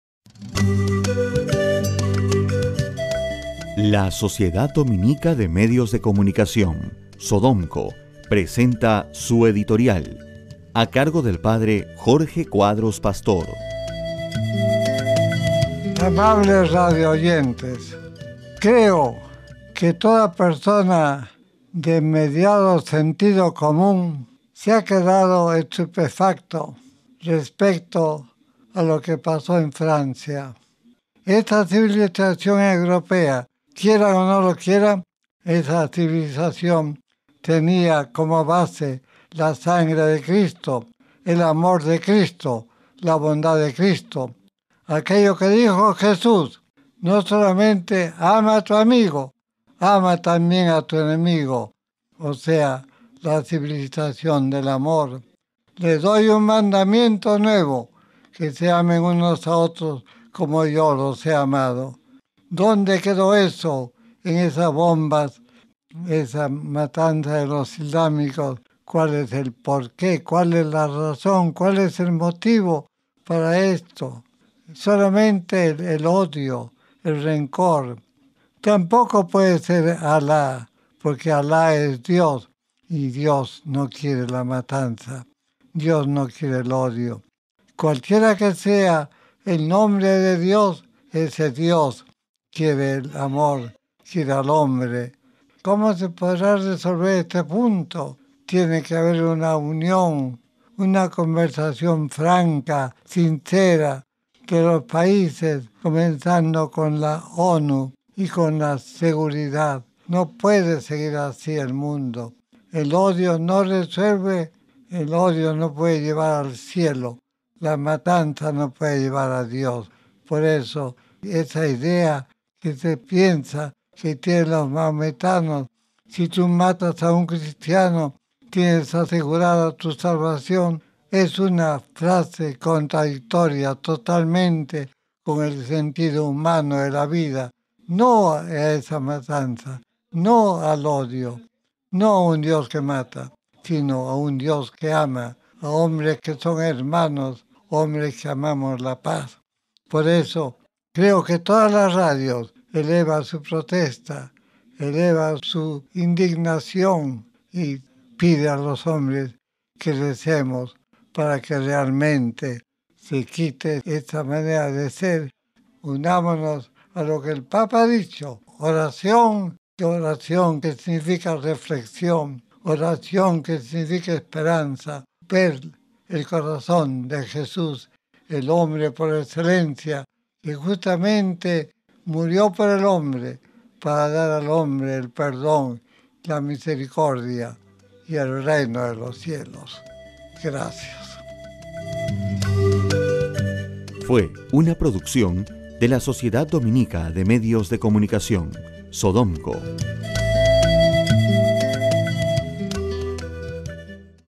Editorial P. Jorge Cuadros (17 noviembre 2015)
Tema: La violencia en el mundo.